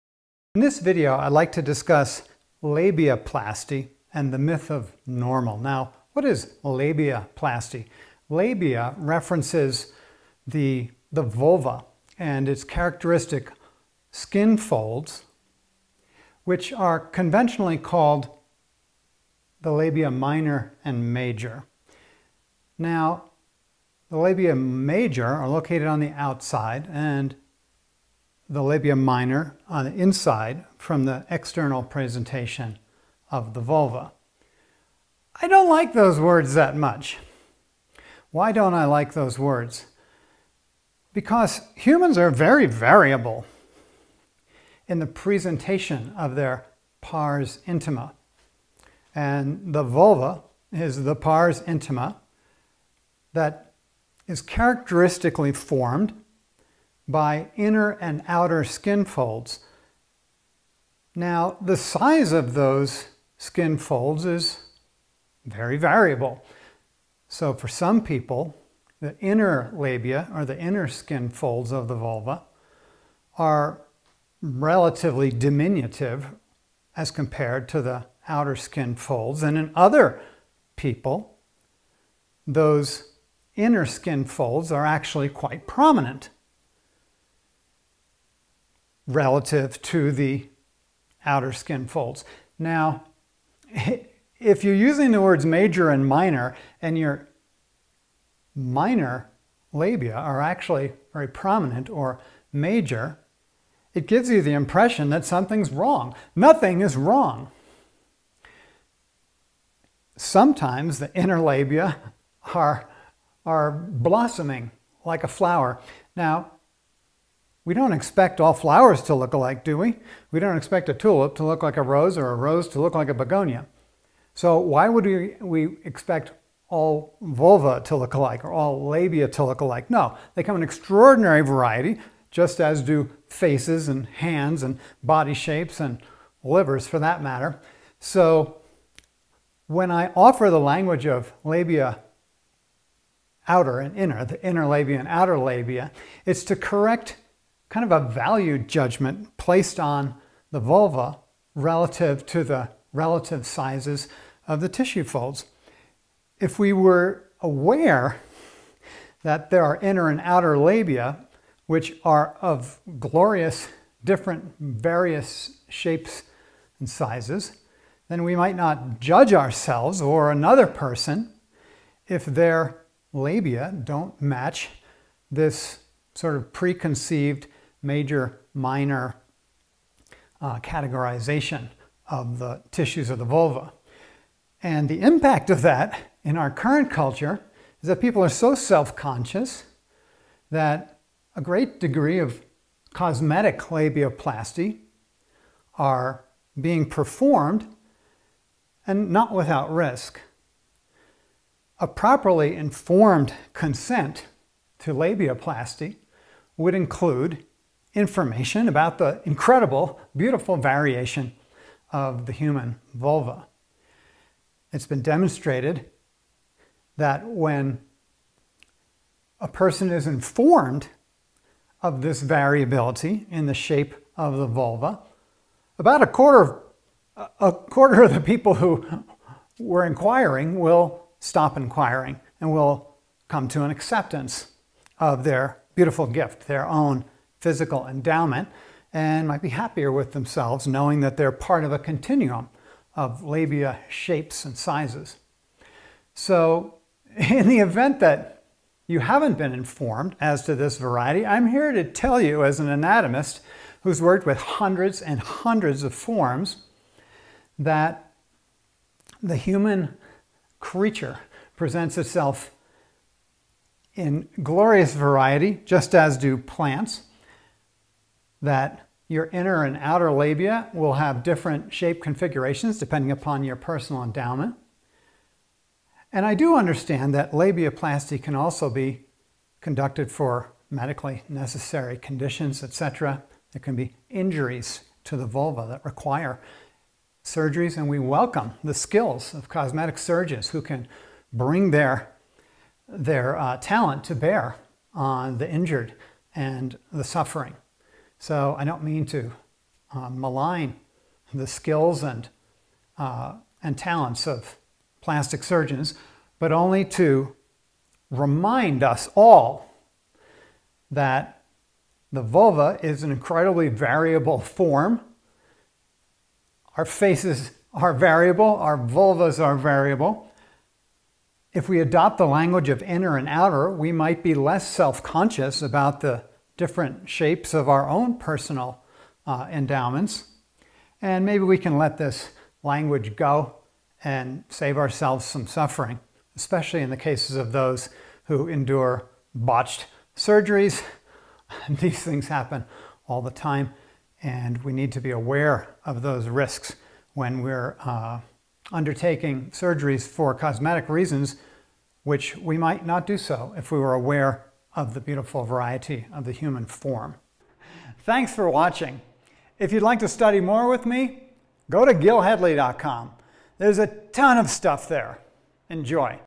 Labiaplasty and the Myth of Normal: Learn Integral Anatomy with Gil Hedley
In this video I challenge the language of labia "major and minor" and offer the option of "inner and outer" labia to correct the judgements of propriety inherent in the established language. I further convey the reality of variation of the human vulva to assure that there is no "right" or "wrong" when it comes to one's personal endowment.
For a detailed accounting of the human vulva, Explorer members of my site can watch Anatomy from A to Z, Courses 2 and 9~ Enjoy!